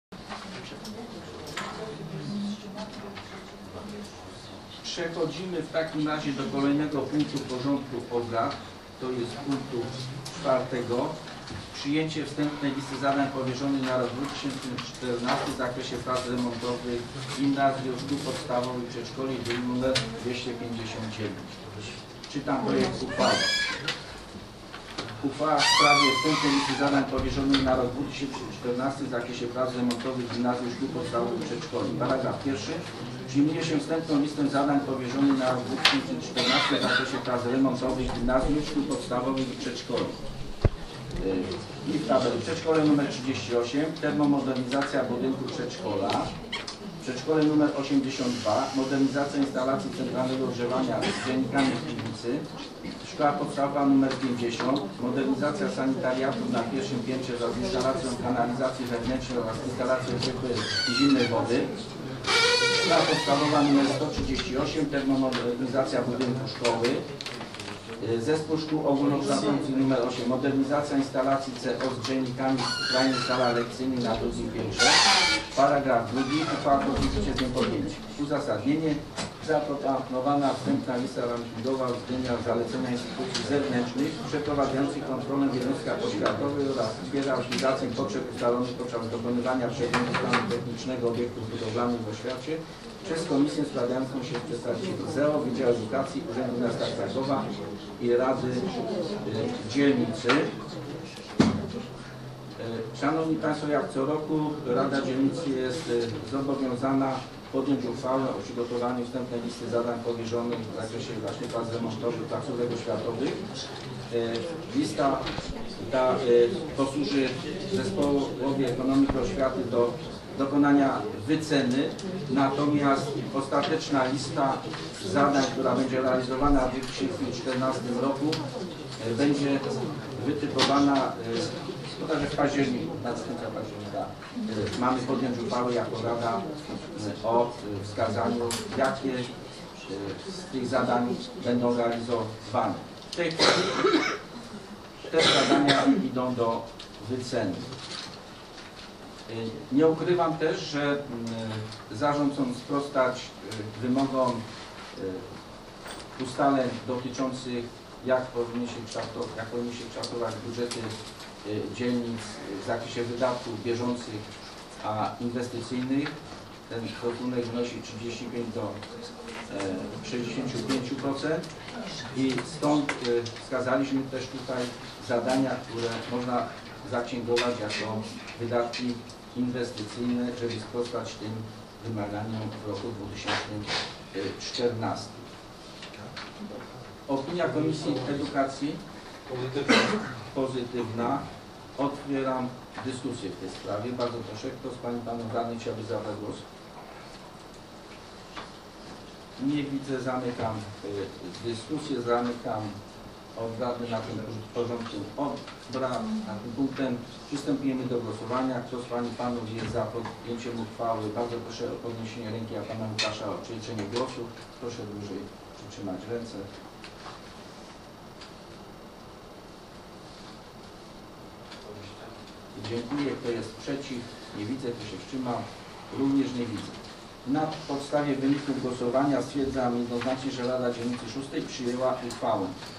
RD_VI_04-07-2013_3 prace remontowe gimnazjów, szkół podstawowych i przedszkoli w 2014 r.
Obrady XXX sesji Rady Dzielnicy VI Bronowice z dn. 4 lipca 2013 r. Uchwała nr XXX/259/2013 w sprawie przyjęcia wstępnej listy zadań powierzonych na rok 2014 w zakresie prac remontowych gimnazjów, szkół podstawowych i przedszkoli.